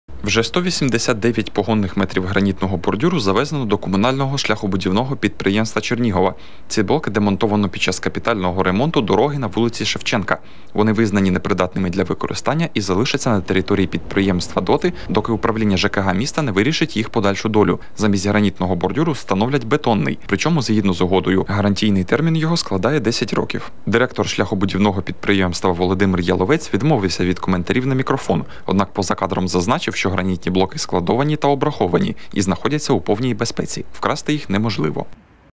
Невловимі бордюри | Телеканал Новий Чернігів
Вже 189 погонних метрів гранітного бордюру завезено до комунального шляхо-будівного підприємства Чернігова. Їх демонтували під час капітального ремонту дороги на вулиці Шевченка.